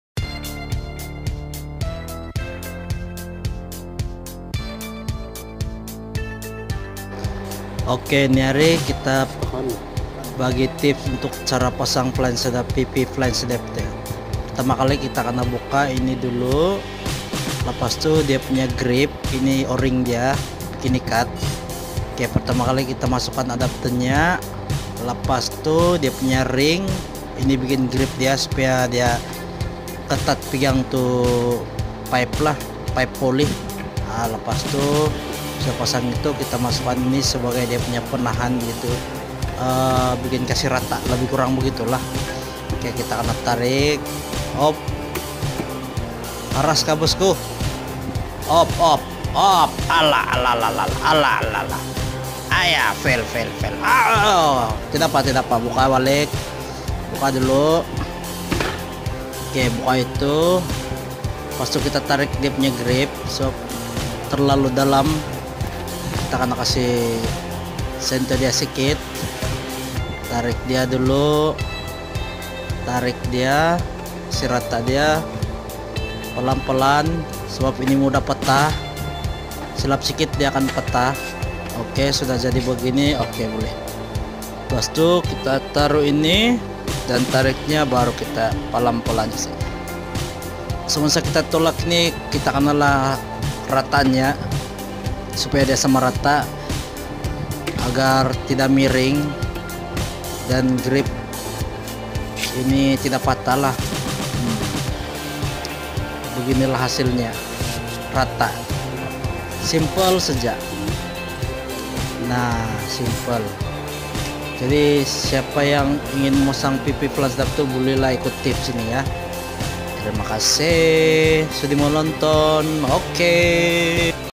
Cara memasang Pe Quick Flange Adaptor
Pastikan kadaan paipe kna rata
.. Pastikan ring tmbanga tda bingkuk
.. Pamasangan kna la hati2.. Dn harus la rata agar ring tmbaga trsebut tda patah..